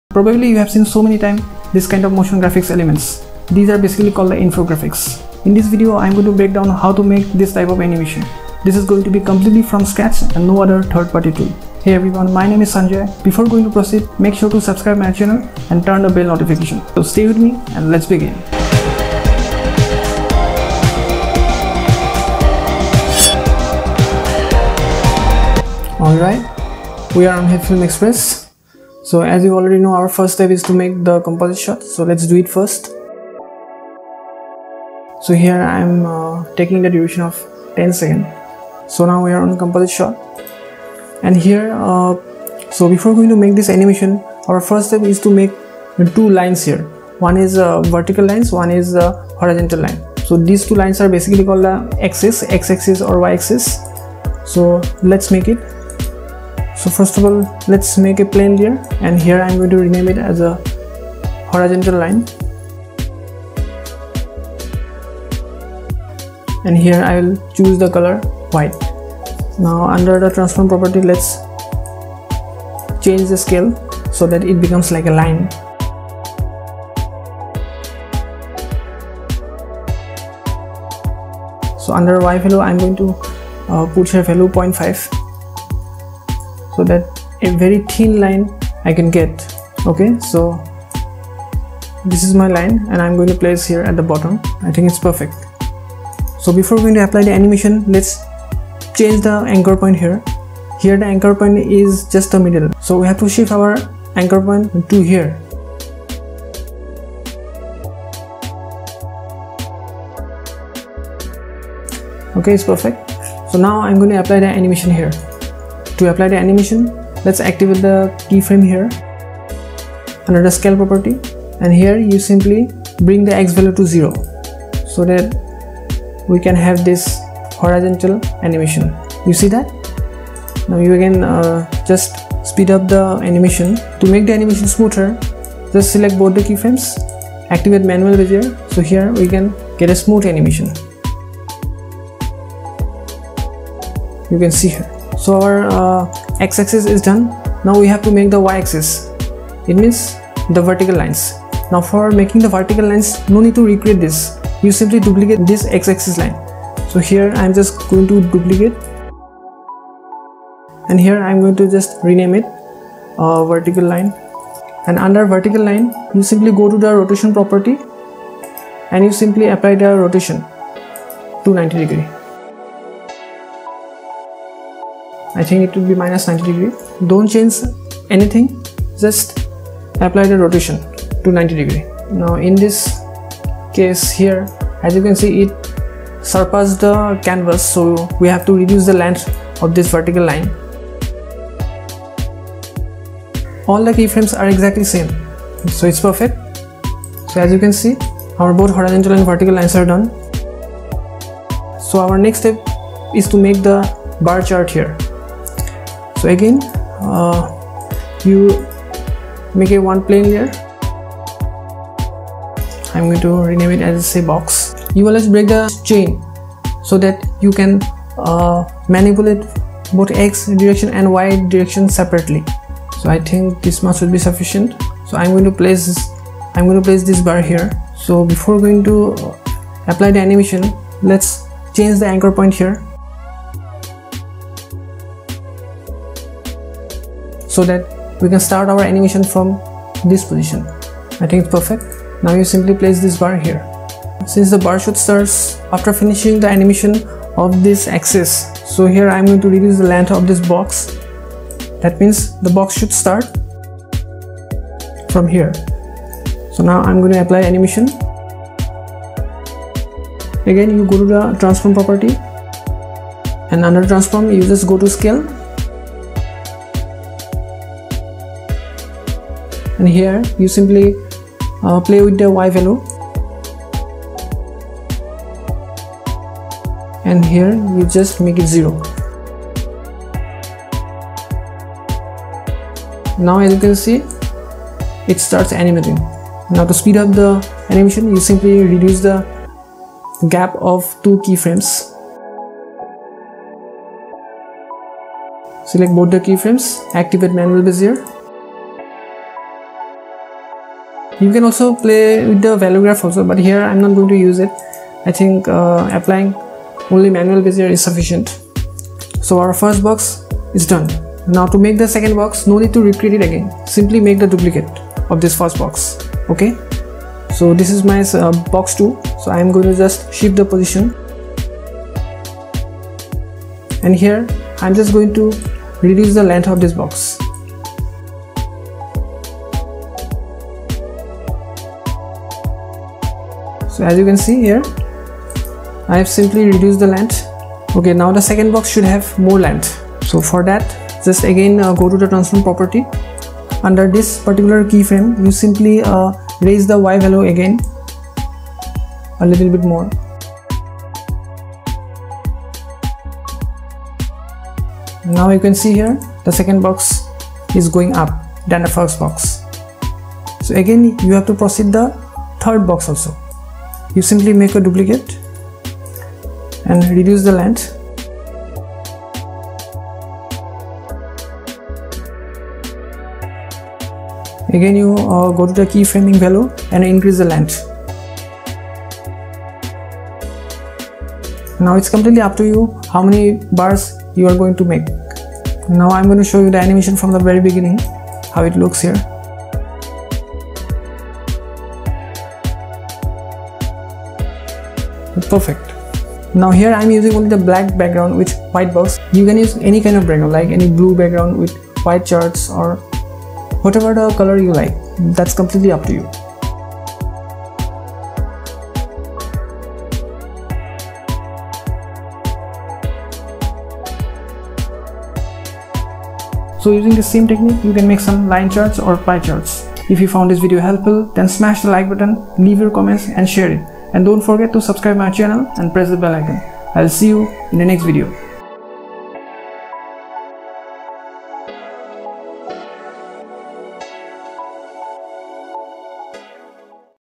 How to make animated charts in hitflm express | Hitfilm animation tutorial
How to make animated charts in hitflm express | Hitfilm animation tutorial | Motion graphics tutorial

Hello,
I am Sanjay. In this channel, I show different types of editing, animation, and VFX-related videos. I hope it will entertain and educate you.

In this video, I have shown you how to make animated bar charts in hitfilm express. This is a complete hitiflm animation  and motion graphics tutorial from scratch